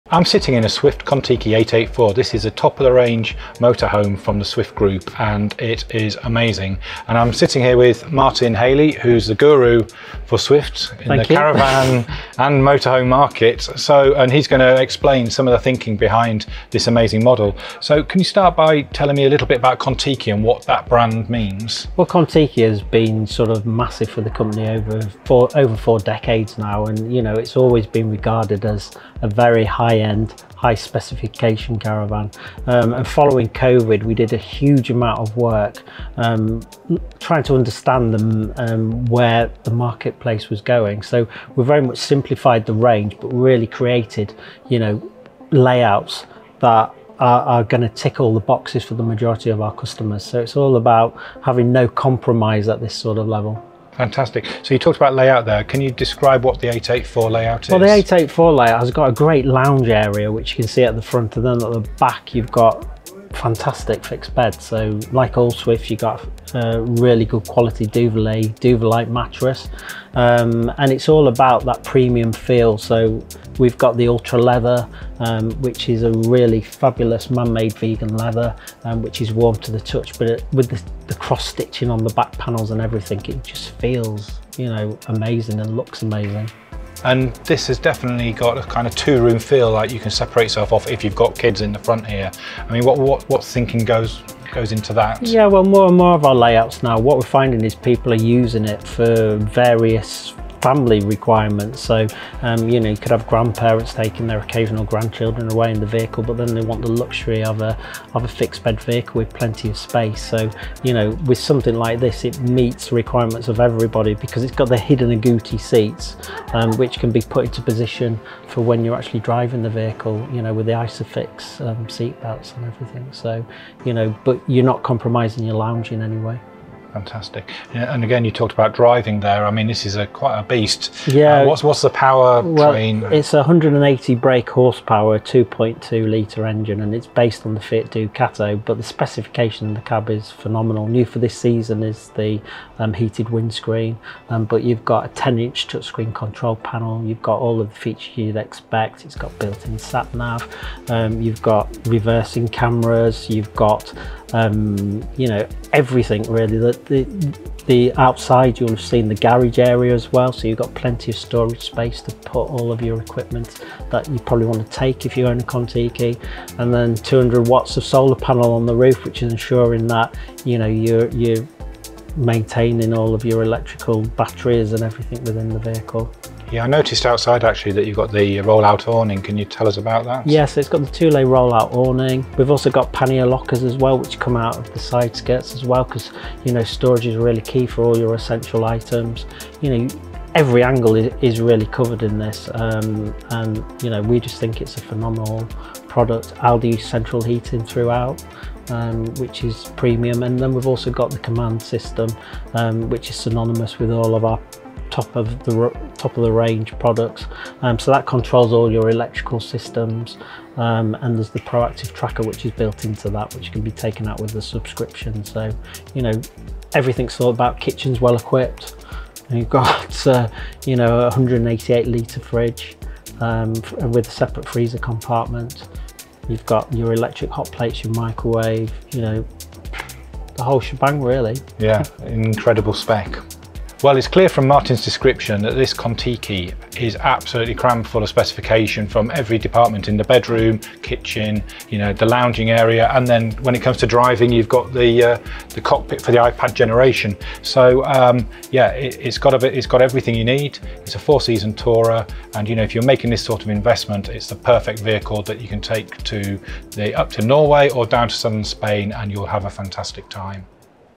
Swift Kon-Tiki 884 First Look | The Ultimate Motorhome?
The Kon-Tiki model sits at the top of Swift's line up of motorhomes and represents the best of what the British brand offers. John Sootheran sits down with Martin from Swift to explore everything this icon has on offer from it's vegan leather seats through to it's Smart construction method and well thought out layout.